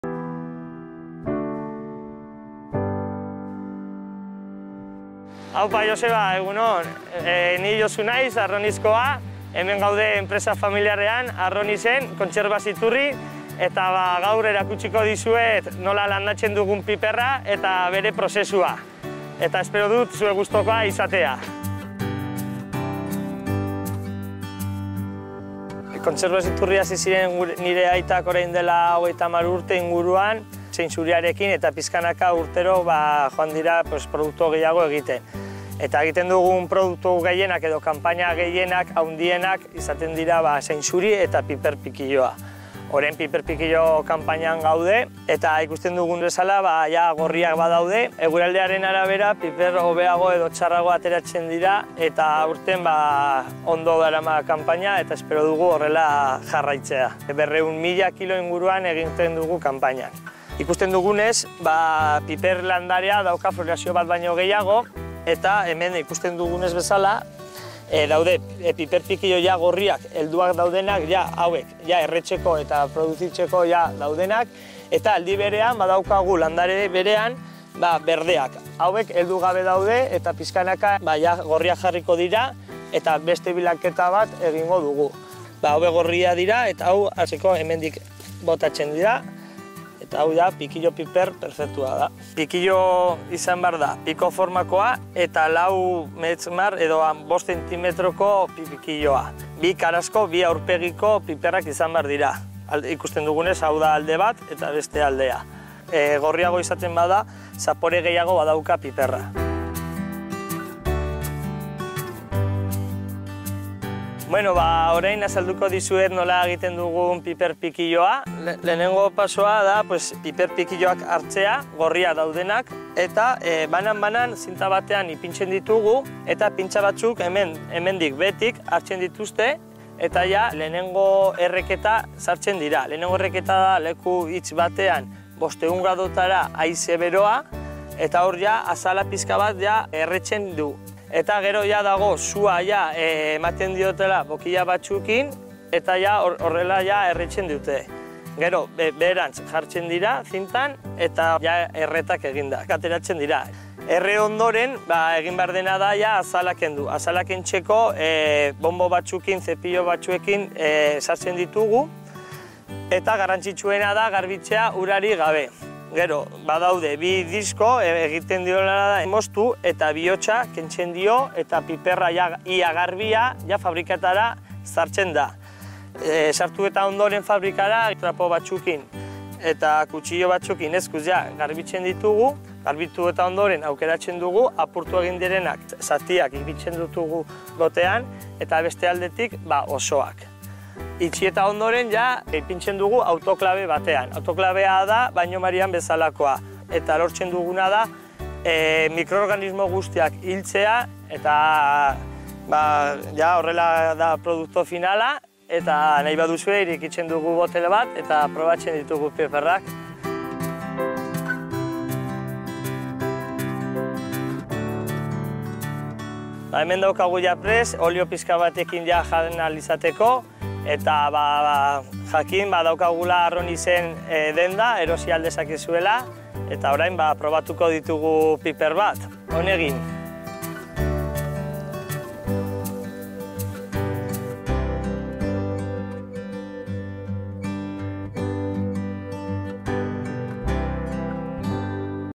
Nola erre eta potoratzen dira pikillo piperrak?
Arroitzeko Iturri Kontserbak enpresan izan gara. Zainzuri eta pikillo piperrak dira bere kanpainarik handienak, eta sortzailearen semeak, Josu Iturrik, piperrak nola potoratzen dituzten irakatsi digu.